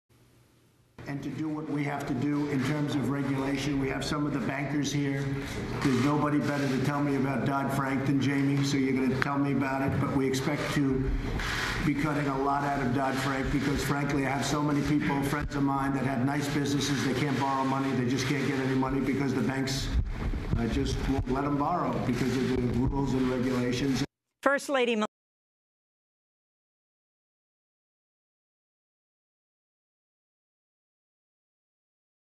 Trump respecto al desmantelamiento de Ley Dodd-Frank: “Mis amigos no pueden pedir dinero prestado”
El presidente Trump reconoció que sus planes para desmantelar la Ley Dodd-Frank de Reforma de Wall Street y Protección al Consumidor están inspirados, en parte, en el deseo de ayudar a sus amigos bancarios. Dodd-Frank es un conjunto de reformas financieras implementadas tras la crisis financiera estadounidense en 2008.